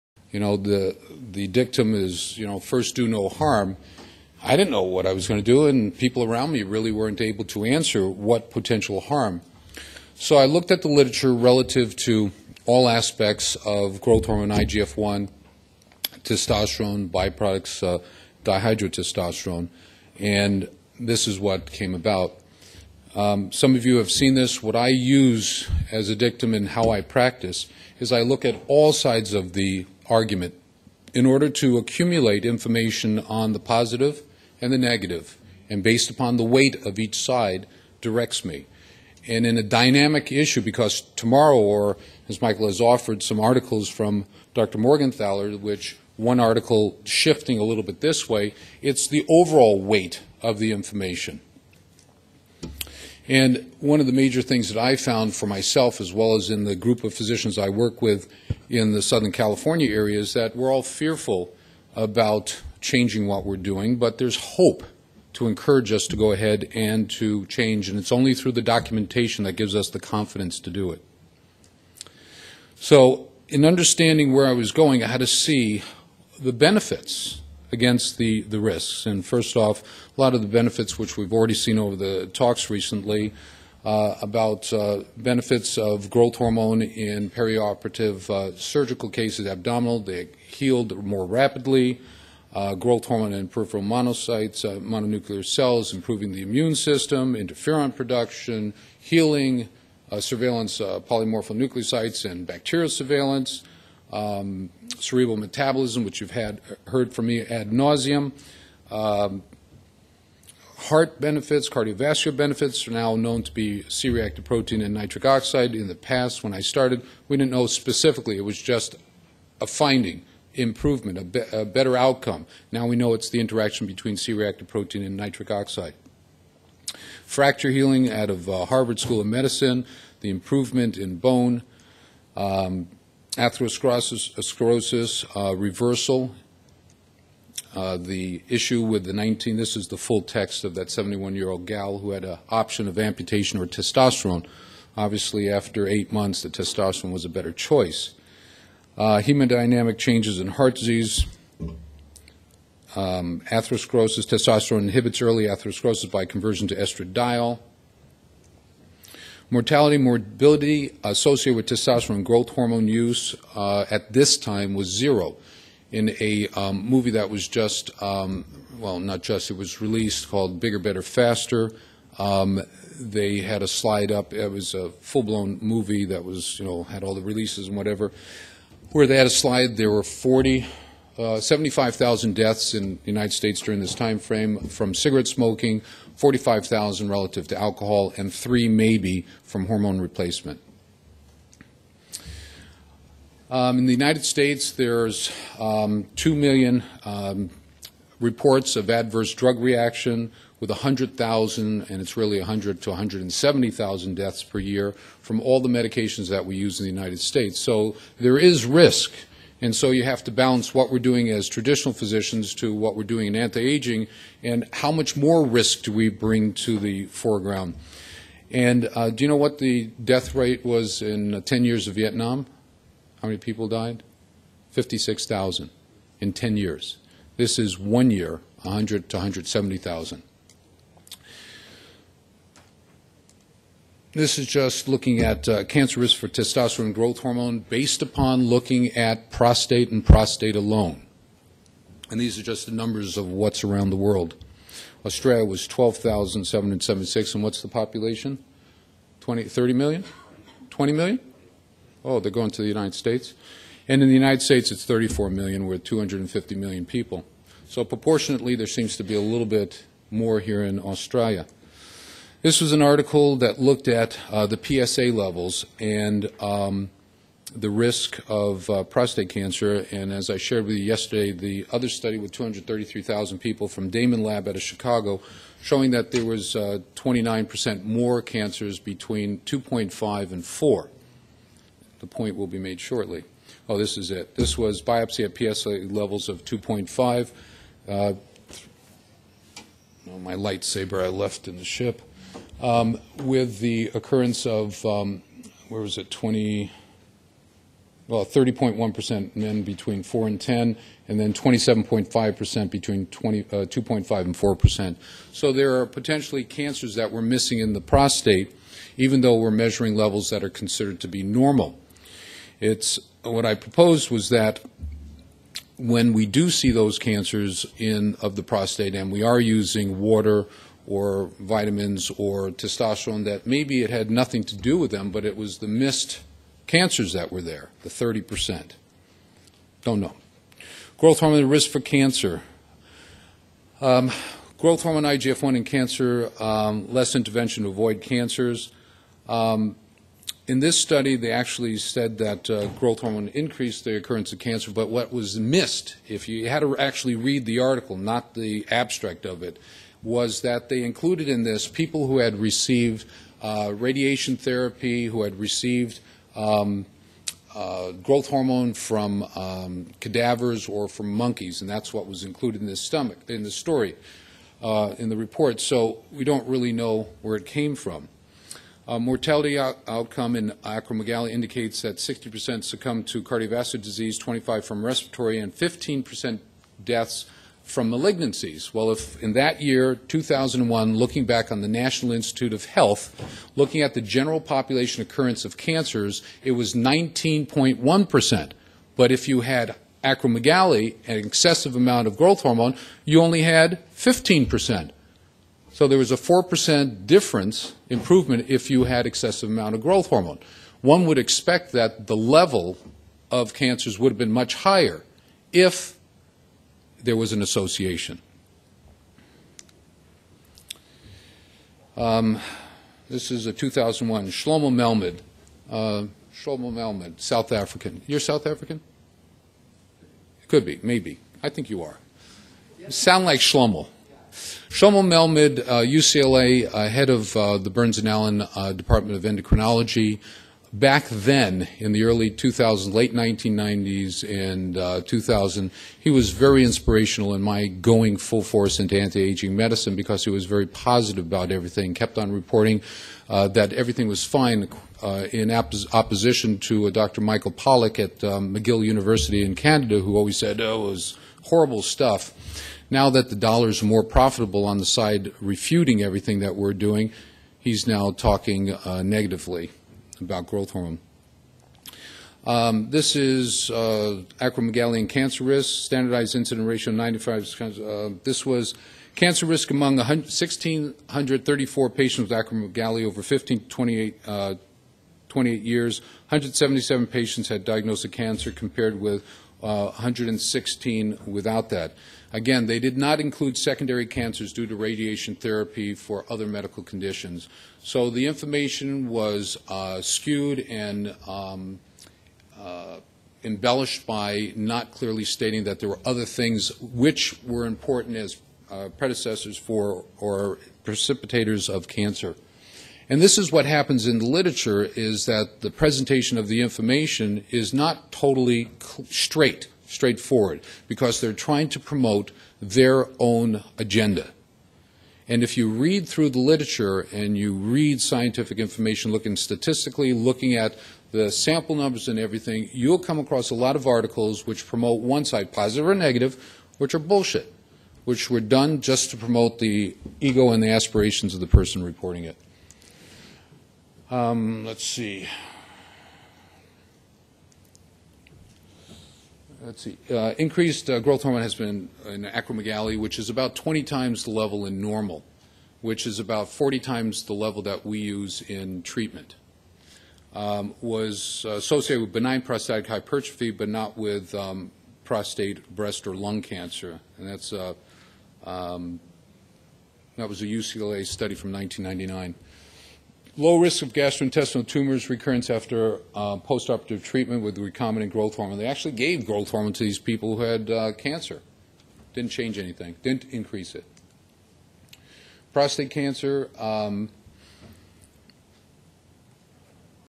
Testosterone, DHT, Growth Hormone and IGF1 and the Potential Risk for Cancer - Dr. Mark Gordon
This is a preview of Testosterone, DHT, Growth Hormone and IGF1 and the Potential Risk for Cancer by Dr. Mark Gordon at the 2008 A5M Australia Conference. to purchase this lecture and many others on DiGiVision, video and slides synced. for the conference sponsor. Item A5M-081W22